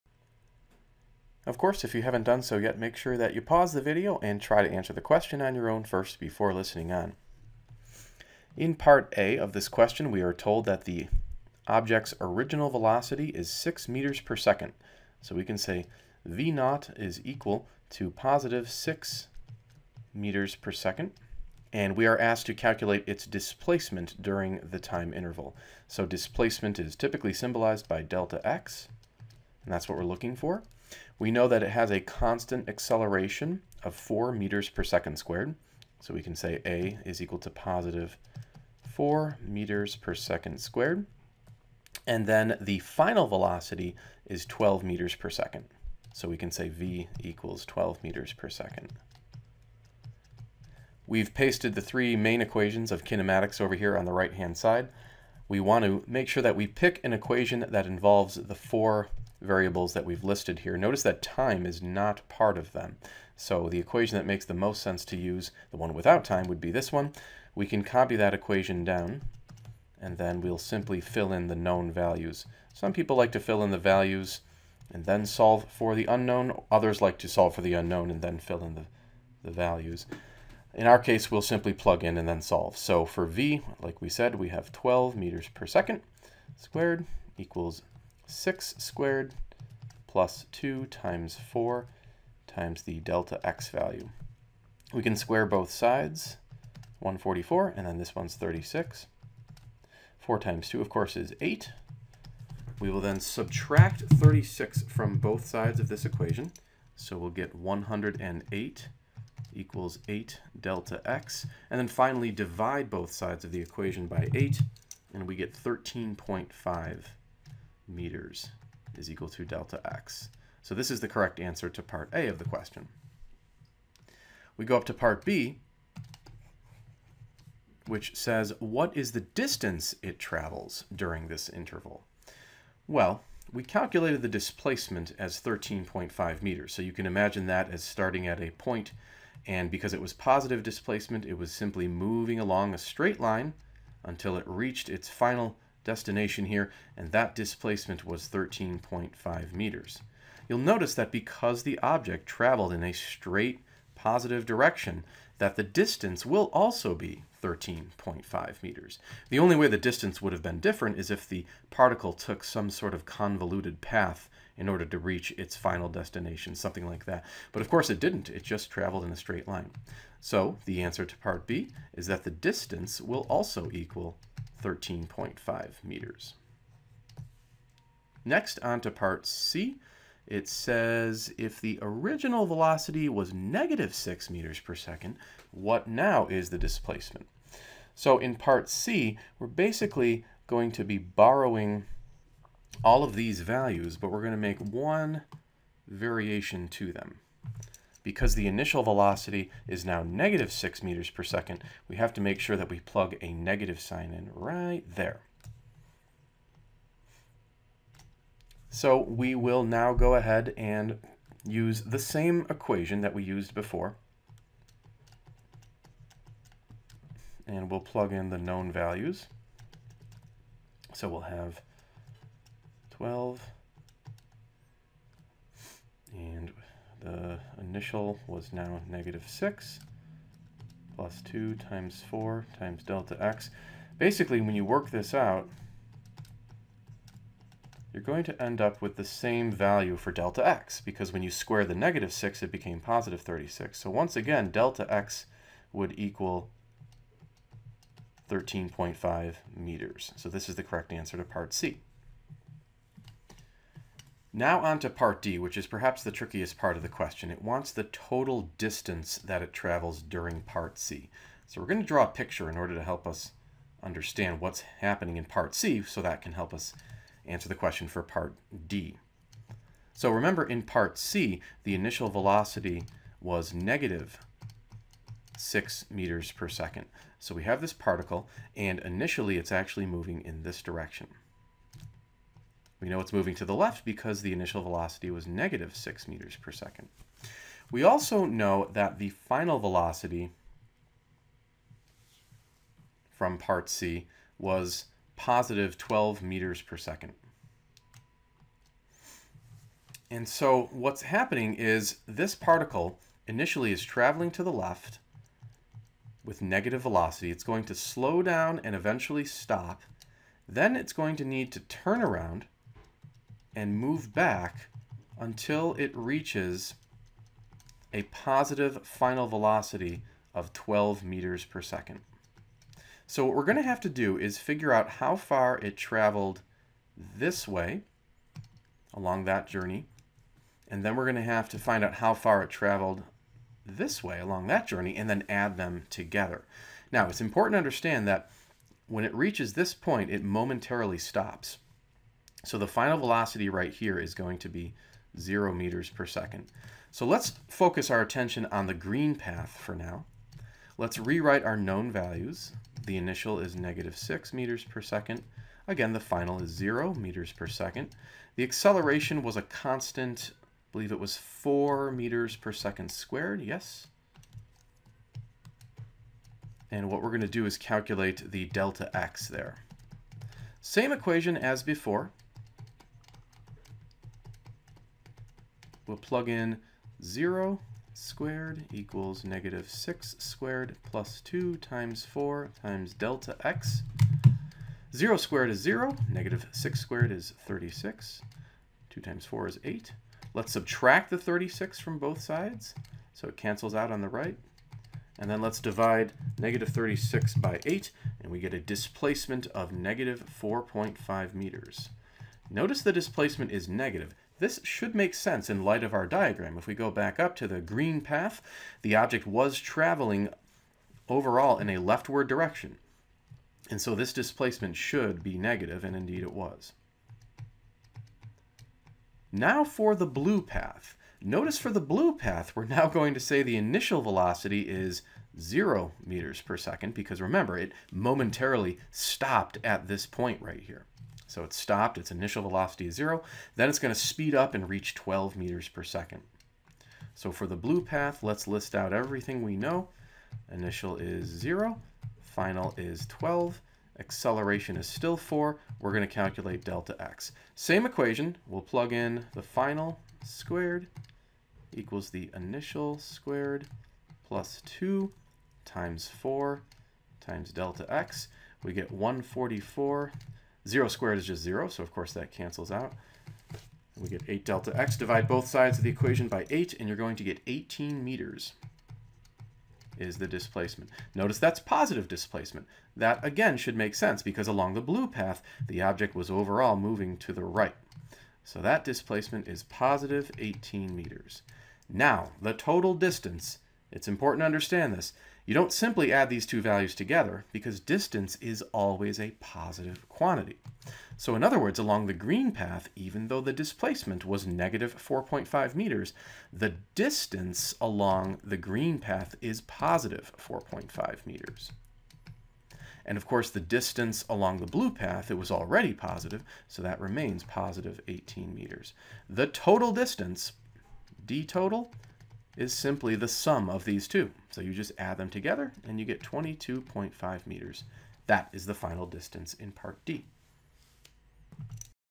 An object moves with constant acceleration 4.00 m/s2
An object moves with constant acceleration 4.00 m/s^2 and over a time interval reaches a final velocity of 12.0 m/s. (a) If its original velocity is 6.00 m/s, what is its displacement during the time interval? (b) What is the distance it travels during this interval? (c) If its original velocity is -6.00 m/s, what is its displacement during this interval? (d) What is the total distance it travels during the interval in part (c)?